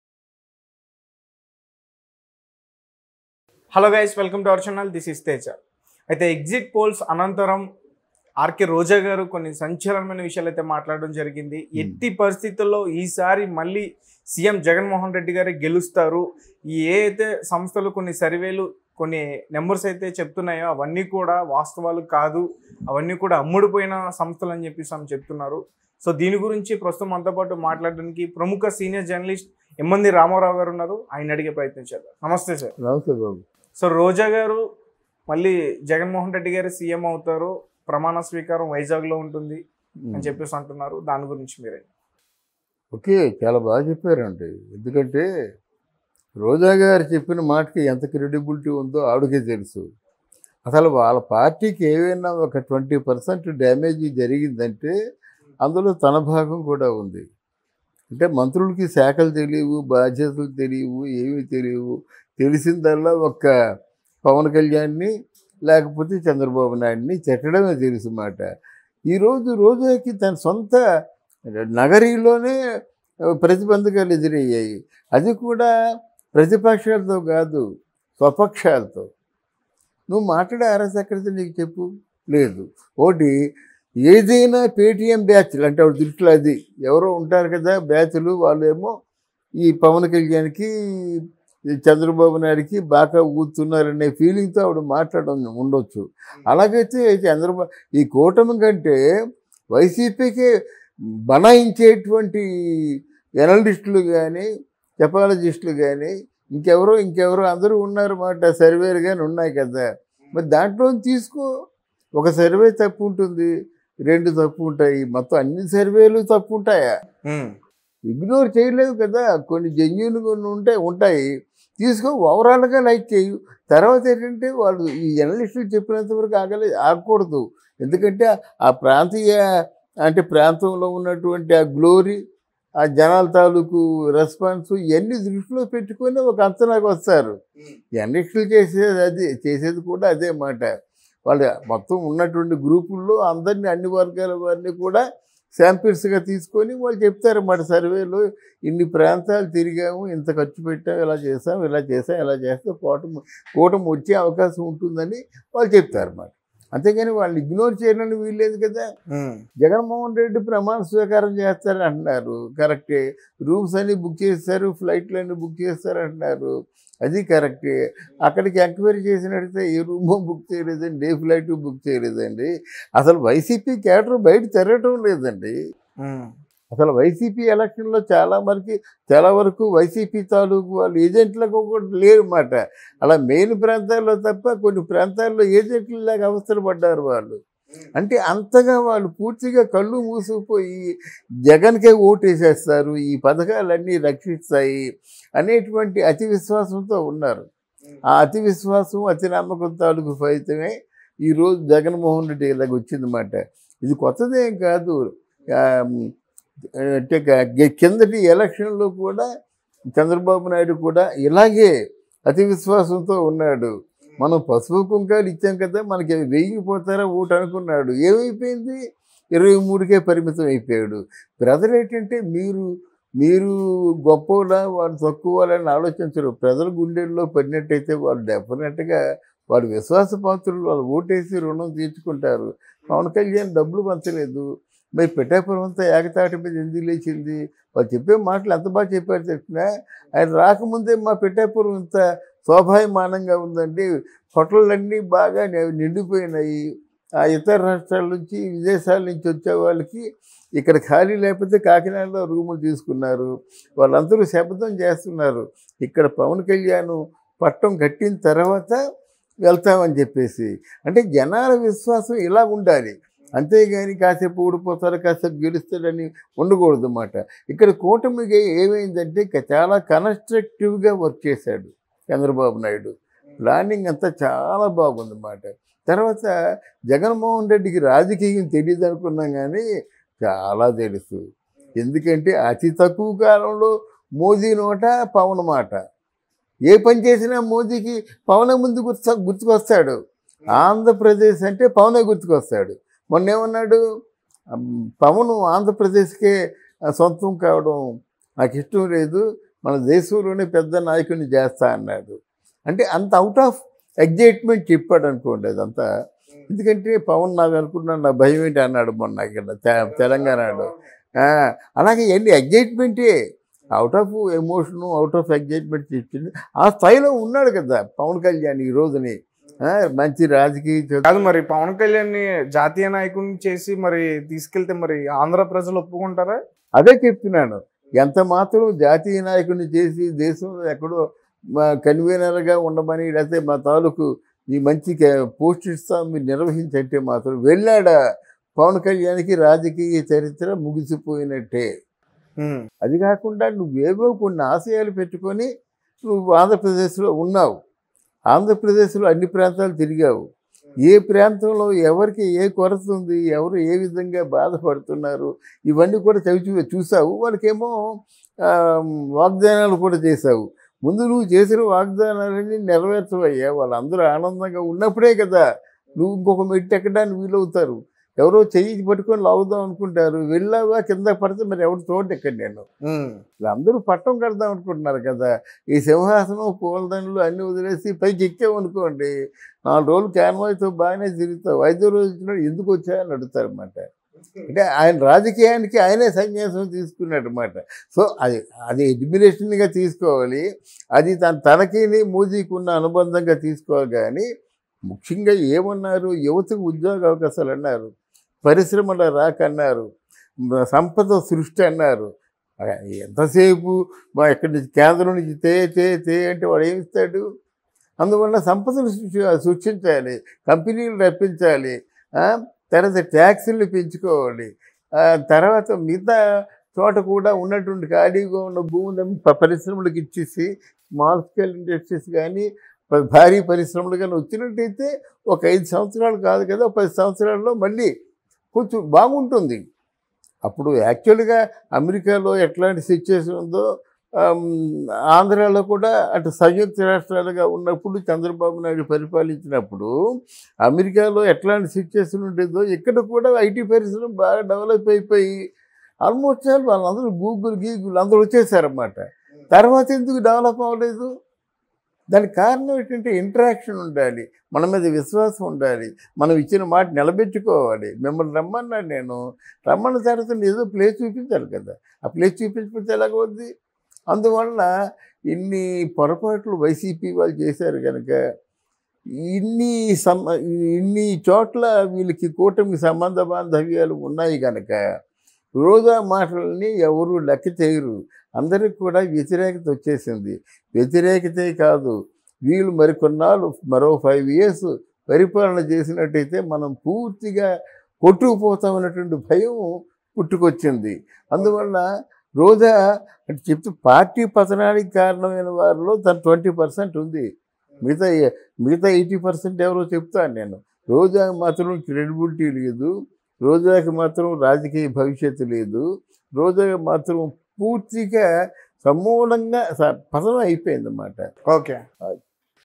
RK Roja BIG Shock To YCP | Roja Video Viral About Exit Poll Results In AP 2024 | Jagan Serious
RK Roja BIG Shock To YCP | Roja Video Viral About Exit Poll Results In AP 2024 | Jagan Serious | RED TV TELUGU